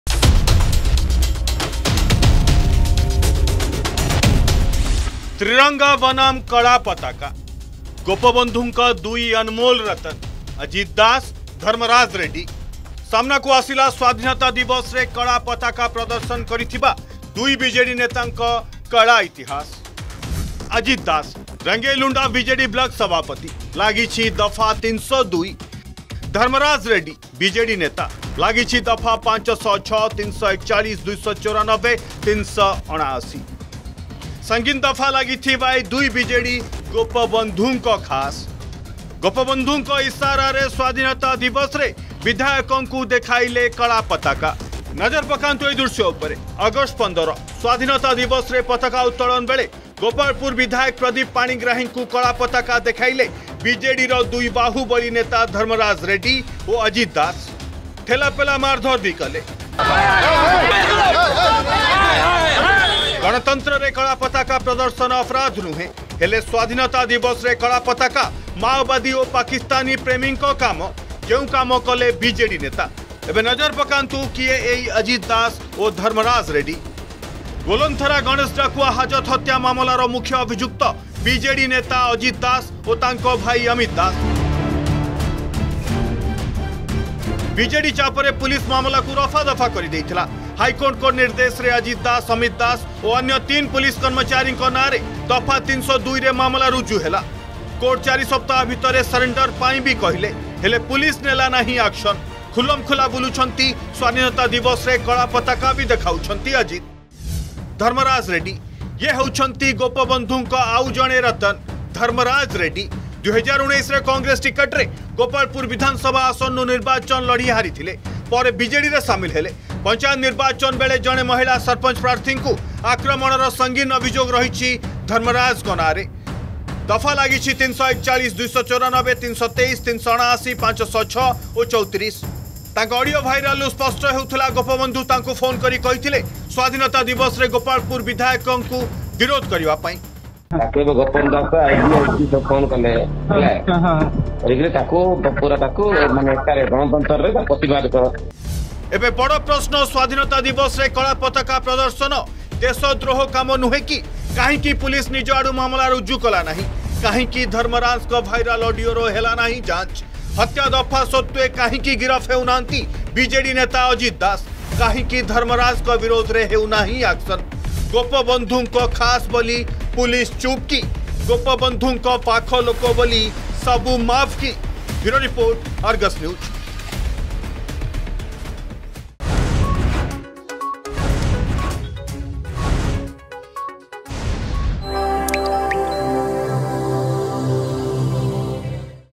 ସ୍ବାଧୀନତା ଦିବସରେ କଳାପତାକା ପ୍ରଦର୍ଶନ କରିଥିବା ଦୁଇ ବିଜେଡି ନେତାଙ୍କ କଳା ଇତିହାସ
ଗୋପବନ୍ଧୁଙ୍କ ଦୁଇ ଅନମୋଲ୍ ରତନ, ଅଜିତ ଦାସ-ଧର୍ମରାଜ ରେଡ୍ଡୀ । ସାମ୍ନାକୁ ଆସିଲା ସ୍ବାଧୀନତା ଦିବସରେ କଳାପତାକା ପ୍ରଦର୍ଶନ କରିଥିବା ଦୁଇ ବିଜେଡି ନେତାଙ୍କ କଳା ଇତିହାସ । ଗୋପବନ୍ଧୁଙ୍କ ଇସାରାରେ ସ୍ବାଧୀନତା ଦିବସରେ ବିଧାୟକଙ୍କୁ ଦେଖାଇଲେ କଳାପତାକା